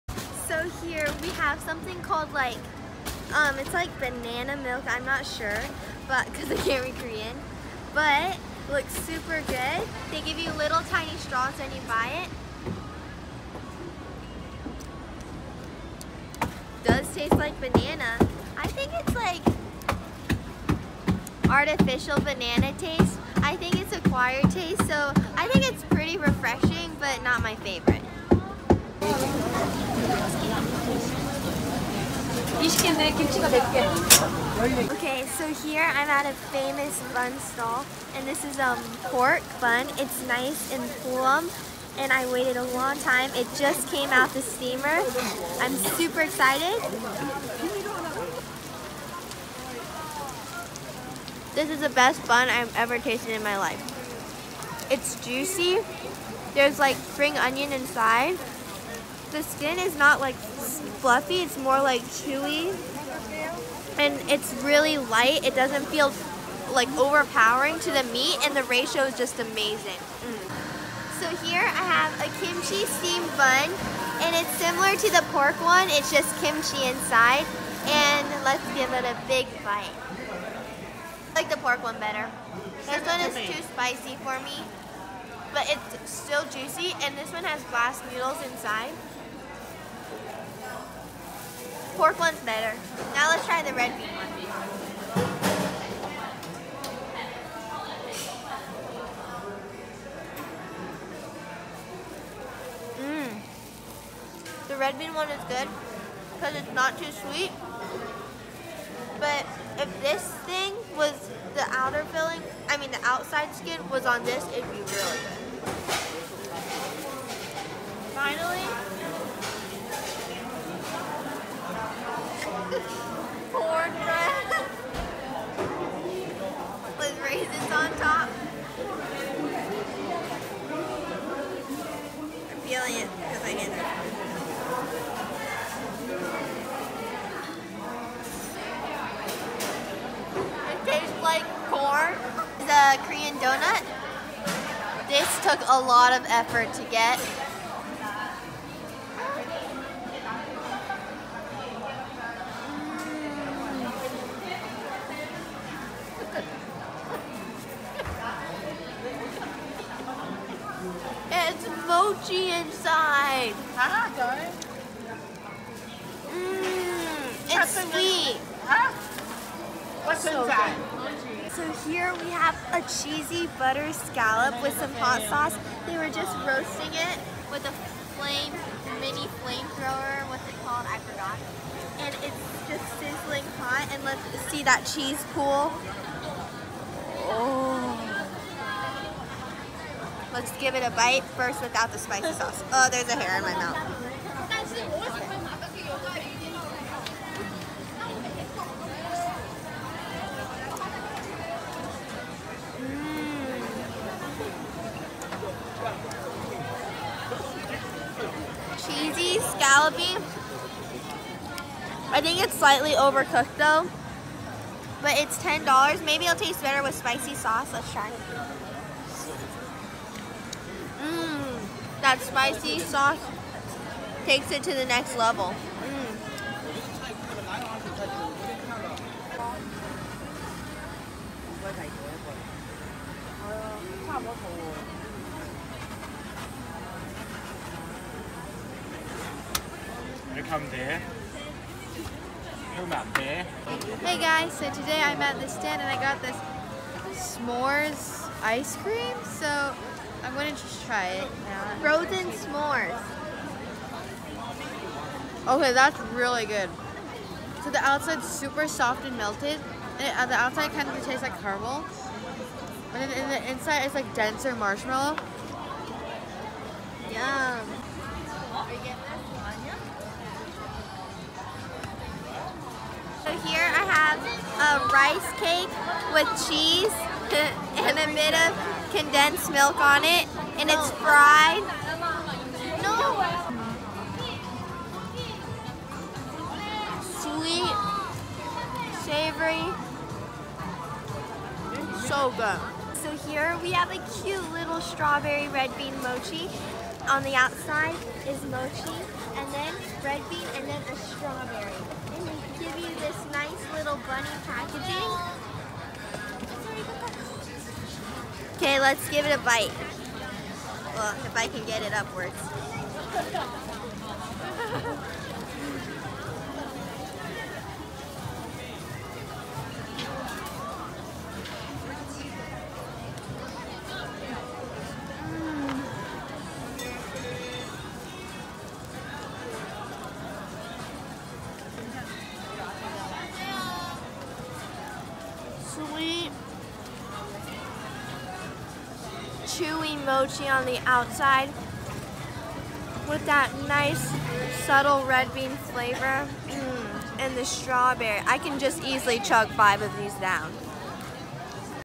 Street Food in Korea, Myeongdong
Here is a low quality video about eating street food in Korea. Everything was super yummy!(except the corn bread lol) Hope you enjoy!